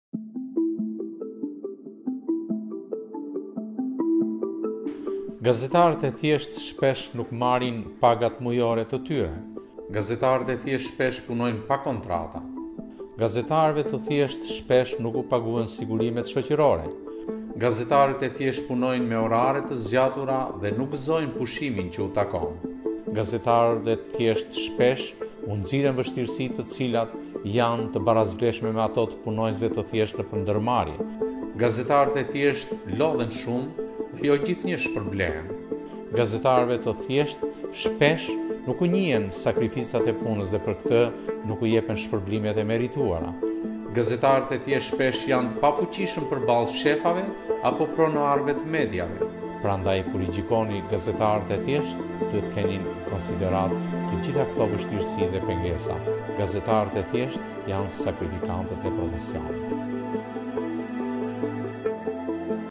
What should we know about labor conditions in the media?
Aleksander Cipa, Chairman of the Union of Albanian Journalists:
Many journalists today work very long hours for very low wages and often without proper contracts and social security protections. This video is about labour conditions in the media today.